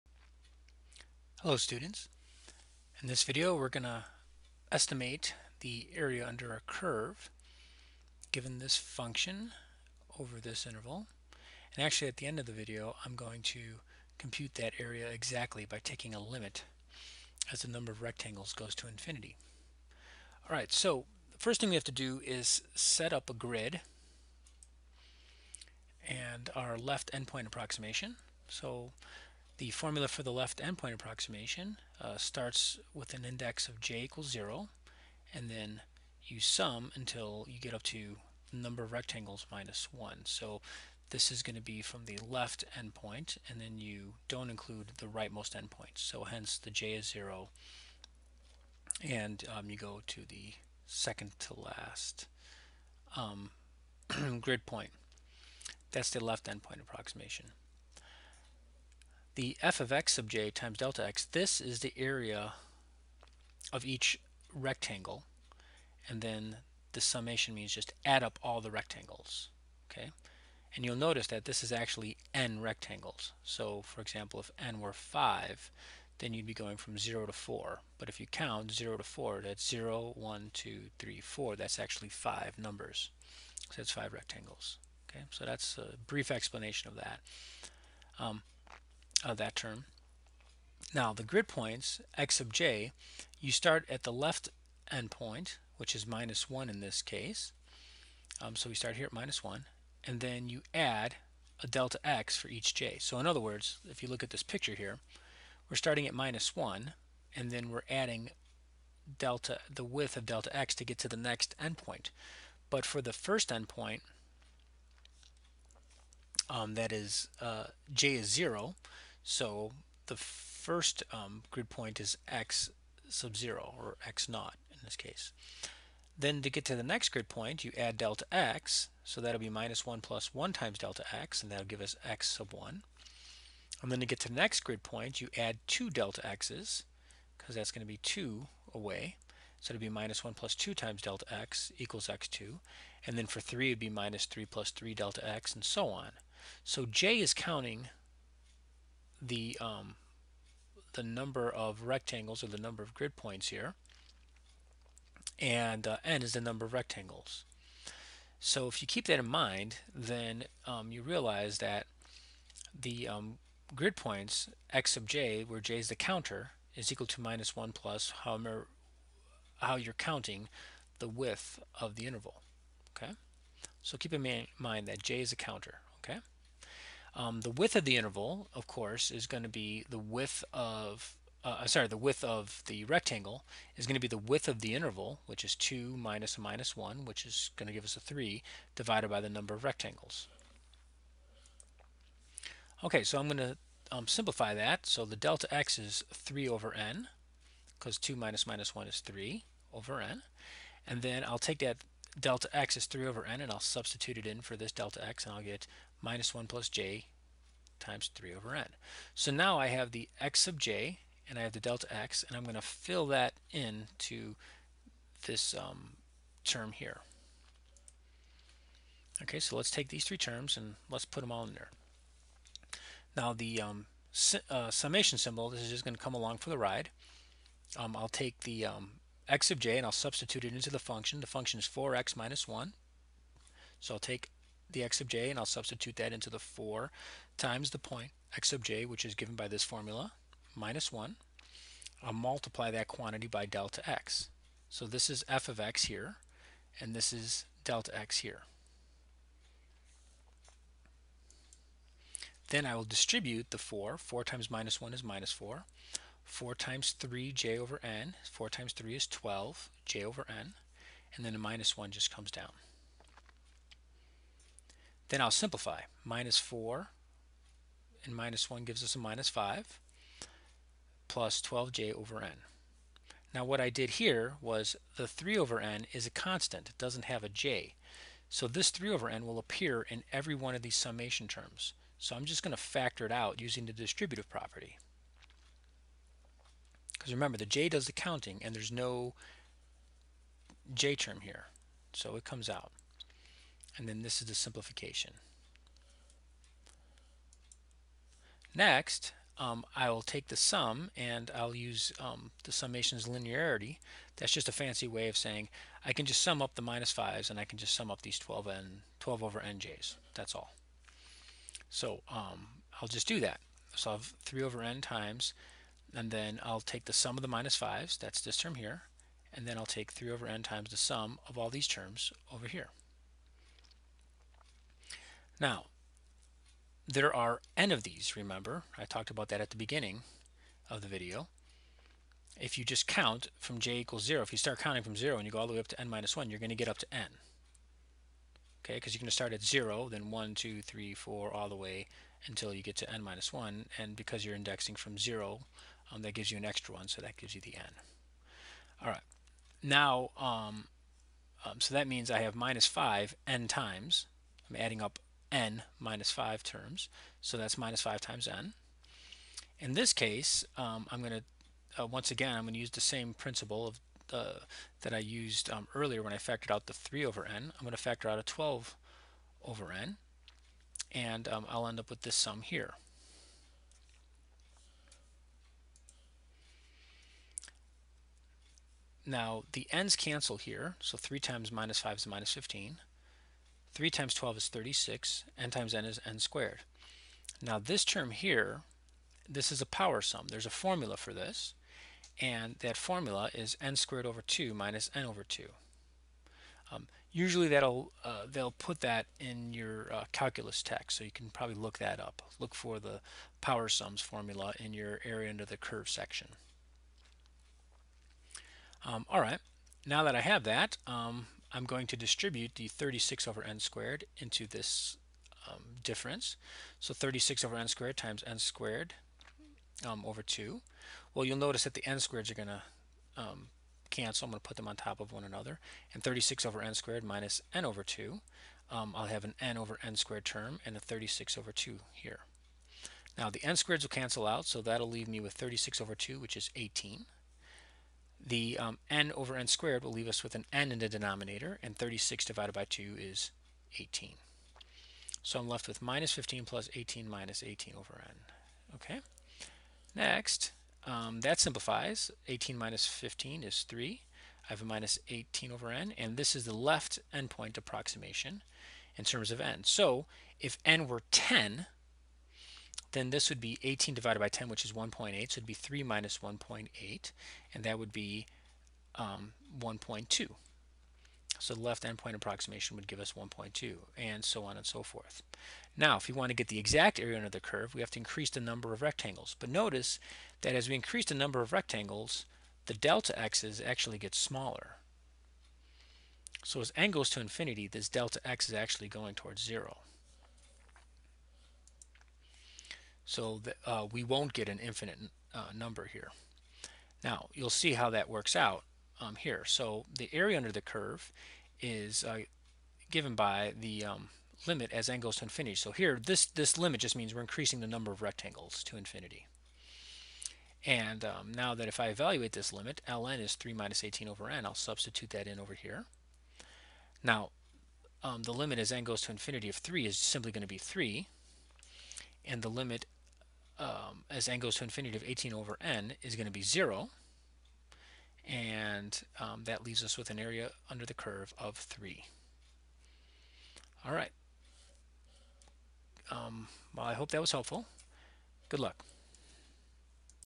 Left endpoint estimate of area under a linear function
Use a left endpoint estimate to compute the area under f(x) = 4x-1 over [-1,2].